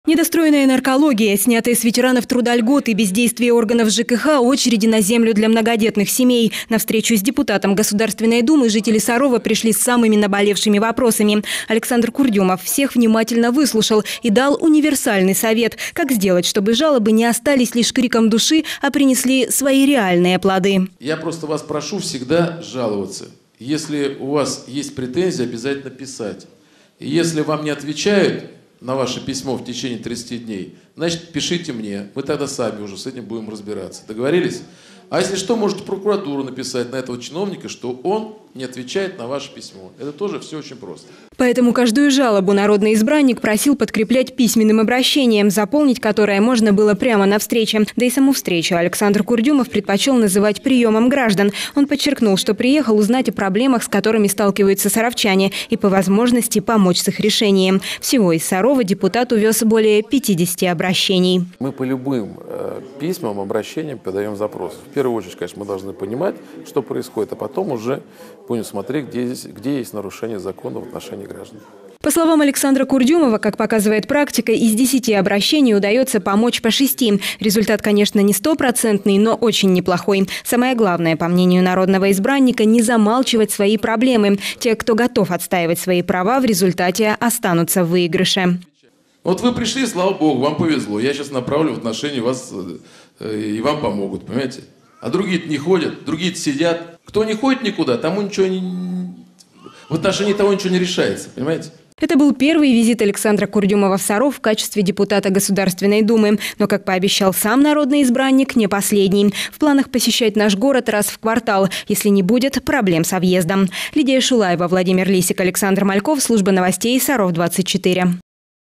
Визит депутата Государственной Думы Александра Курдюмова в Саров
Депутат Государственной Думы Александр Курдюмов посетил Саров. В программе рабочего визита, в том числе, и встреча с горожанами, приглашая на которую, народный избранник призывал саровчан не молчать, а жаловаться.